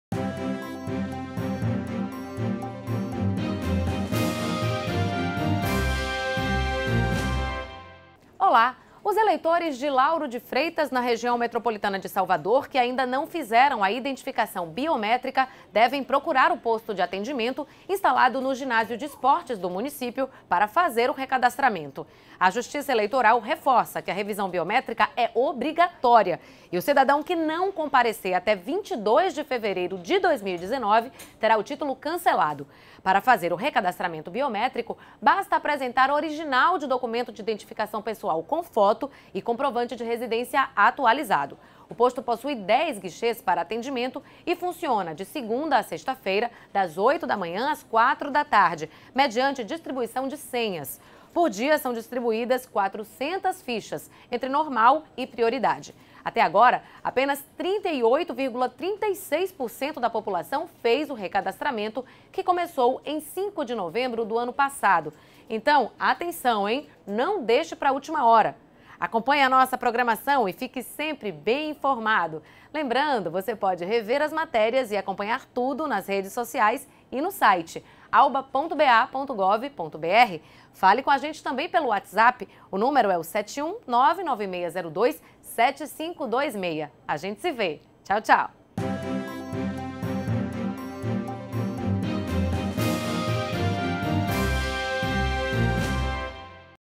TV ALBA Informa - Recadastramento Biométrico
Os moradores de Lauro de Freitas, região metropolitana de Salvador, que ainda não fizeram a identificação biométrica devem procurar o posto de atendimento instalado no ginásio de esportes do município para fazer o recadastramento. A Justiça Eleitoral reforça que a revisão biométrica é obrigatória e o cidadão que não comparecer até o dia 02 de fevereiro de 2019 terá o titulo cancelado. Para fazer o recadastramento biométrico é preciso levar documento de identificação original com foto e comprovante de residencia atualizado.